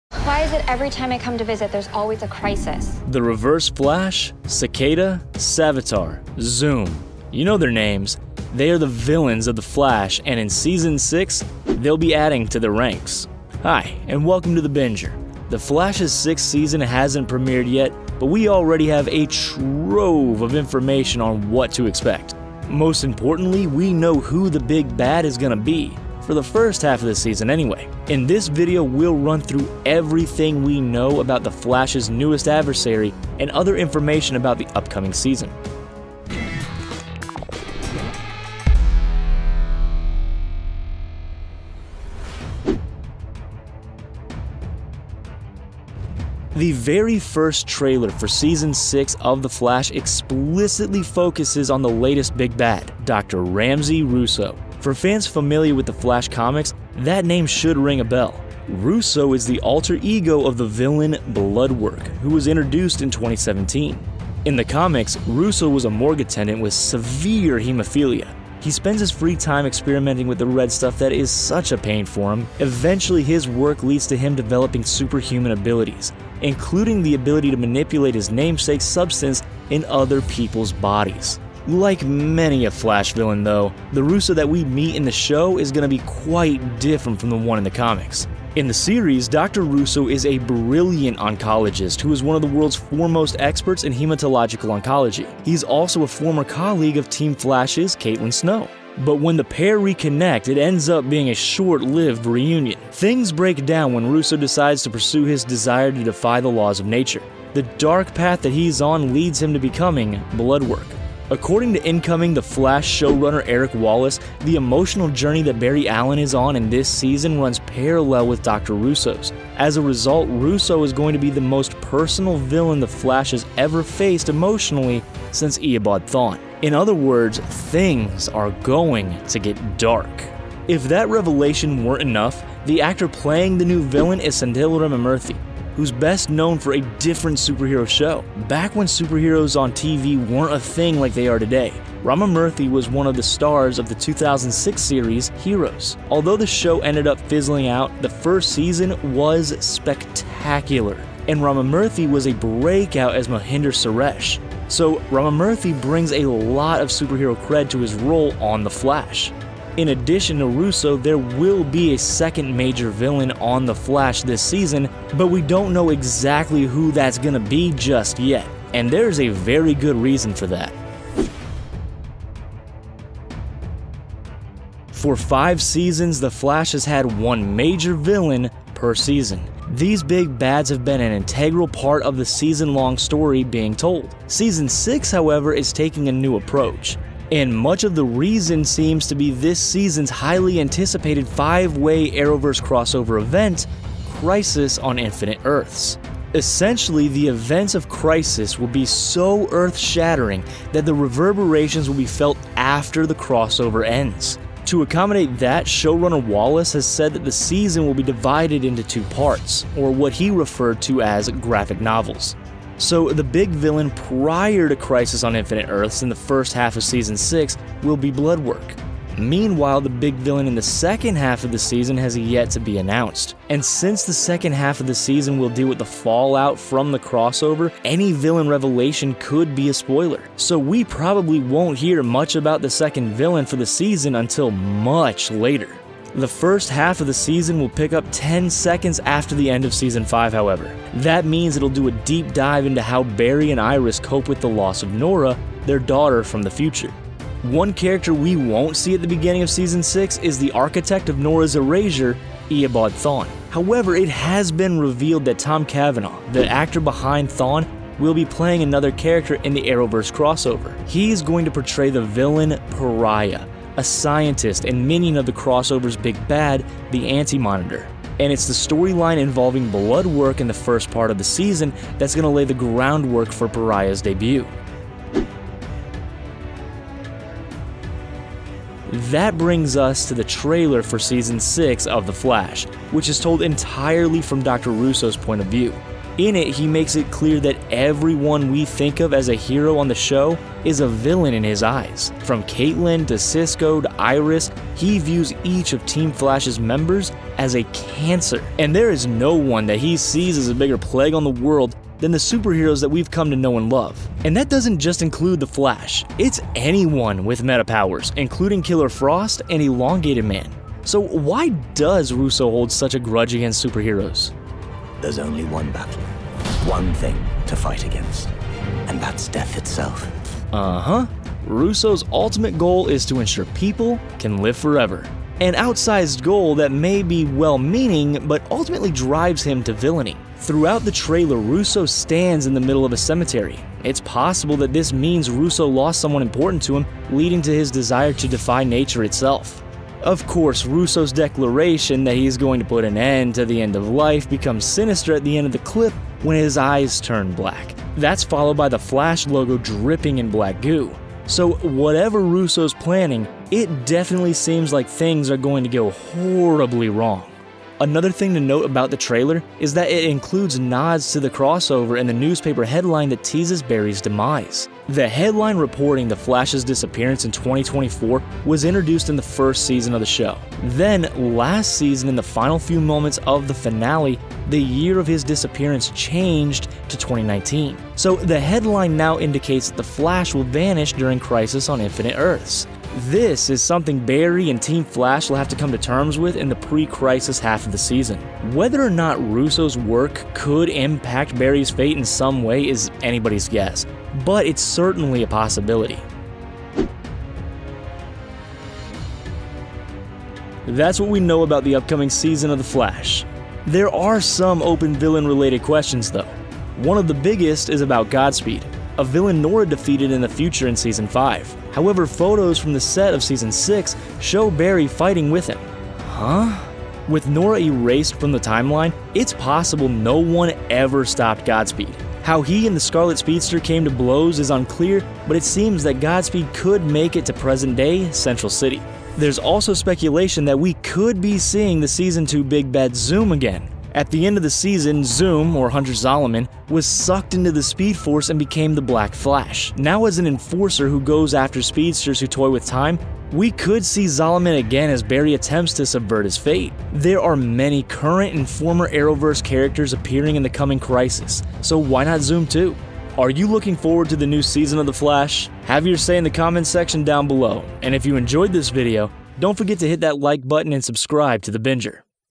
The Flash Season 6 Villains Confirmed And New Information
The Flash Season 6 Has A Lot Of Leaked News And Spoilers

With the highly anticipated crossover event “Crisis on Infinite Earths” coming, fans have been keeping a close eye on any and all information that comes out about the shows of the Arrowverse. Recently, interviews and a new trailer have revealed juicy information about The Flash, including the big bad for the first half of the season Dr. Ramsey Rosso, aka Bloodwork.

In this video, we’ll share what we know so far about The Flash’s upcoming sixth season. We’ll fill you in on exactly who the new villain is and the actor playing him. We’ll also explain what the villain’s appearance will mean for season 6. Then we’ll dive into the new trailer for the season. We’ll explore what the trailer says about this latest villain and his perspective on our heroes. We’ll also touch on the way the first half of season 6 will lead into the upcoming crossover. Finally we’ll wrap up with a couple theories about additional villains, like Godspeed, we just might see in season 6. Hit play and we’ll have you up to speed in no time.